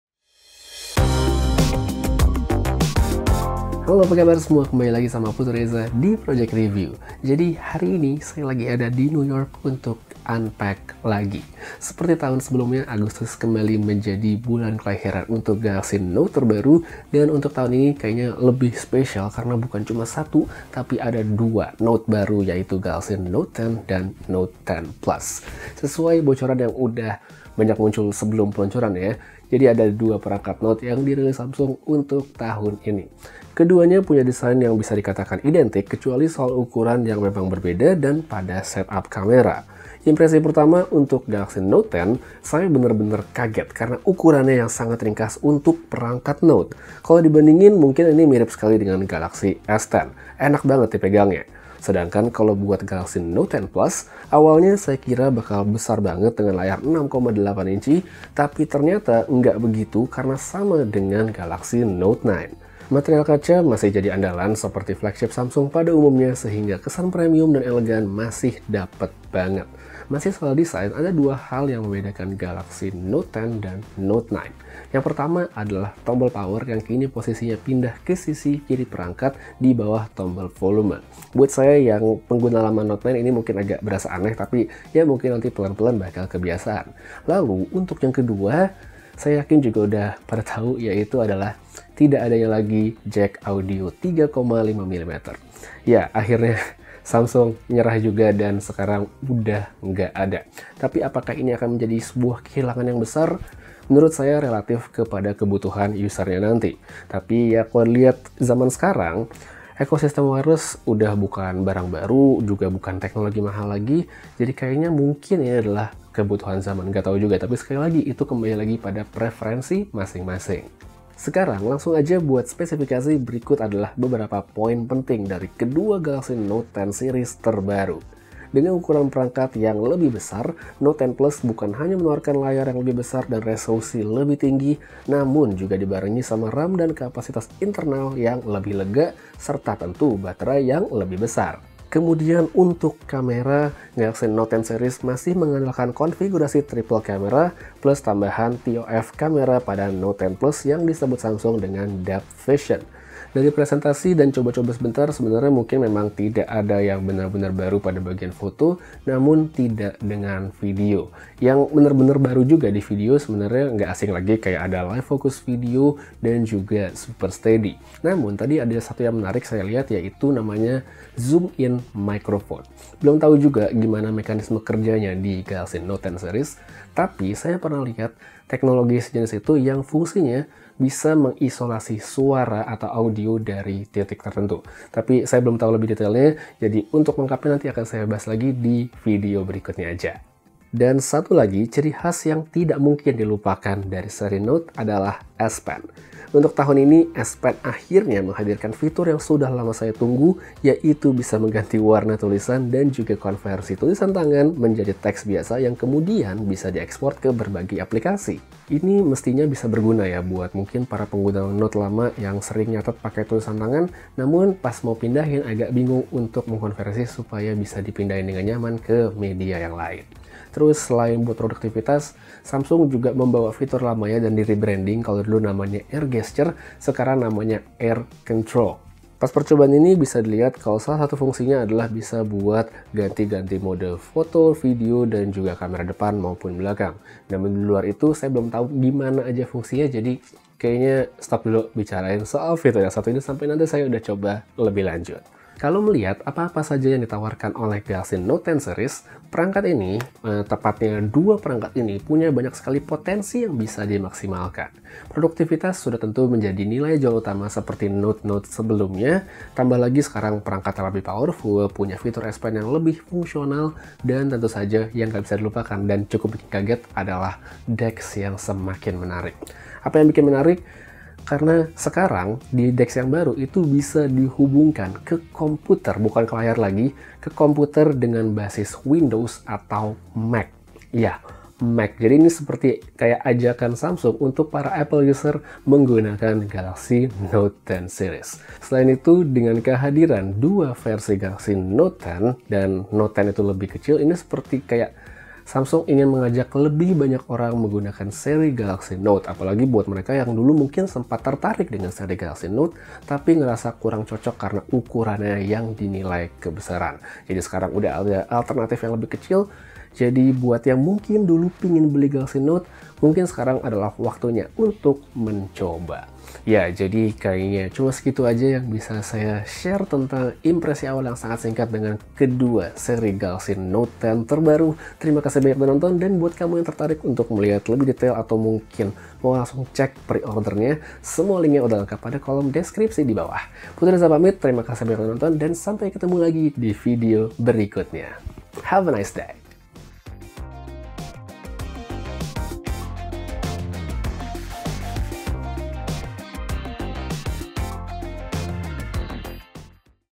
Apa Aja yang Baru di Samsung Galaxy Note 10?
2019 tahun yang special karena bukan cuma satu, tapi ada dua Galaxy Note. Ini dia Samsung Galaxy Note 10 dan Note 10+.

Harga-harganya:

Samsung Galaxy Note 10 | 8/256GB: Rp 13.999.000

Samsung Galaxy Note 10+ | 12/256GB: Rp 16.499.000

Samsung Galaxy Note 10+ | 12/512GB: Rp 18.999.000